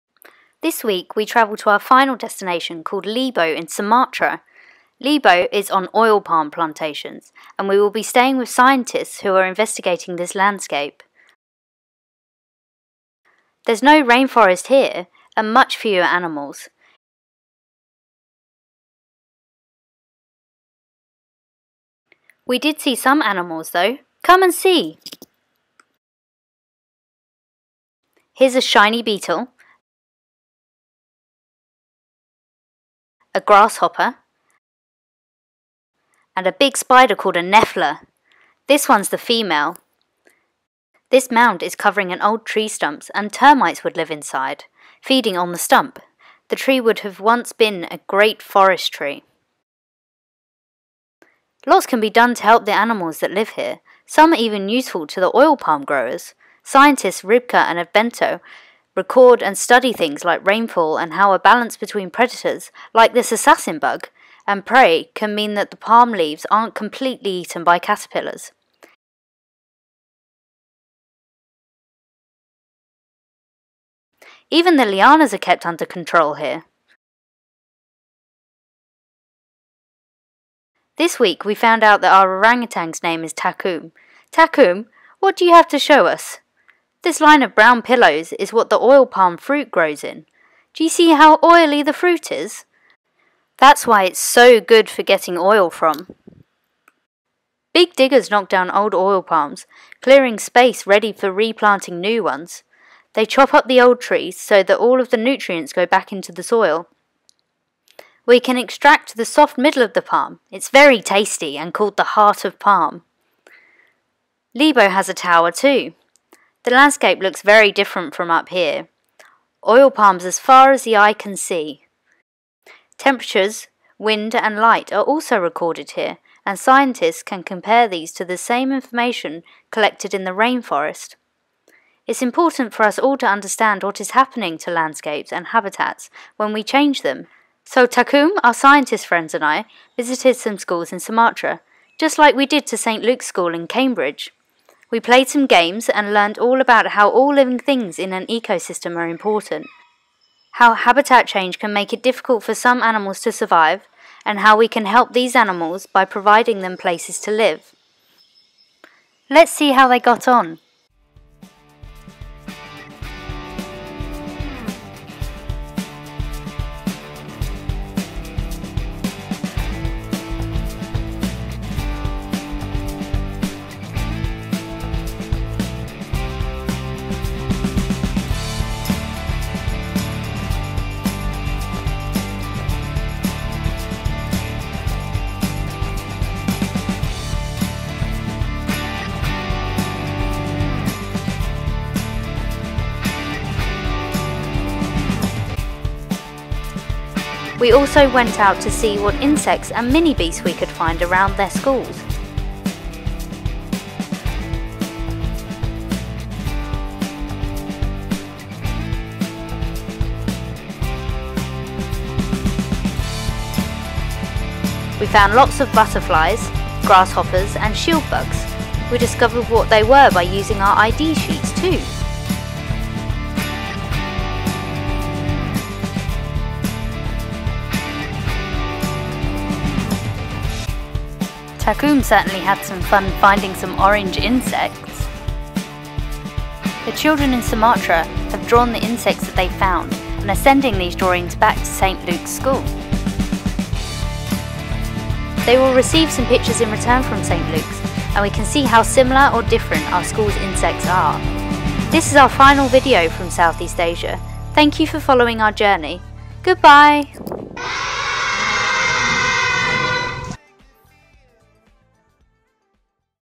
Across the Continents Project: The Next Generation
The Across the Continents Project explores habitat change with curriculum links. Exploring the consequences for our world and introduces the current Department of Zoology research and fieldwork that hopes to combat this.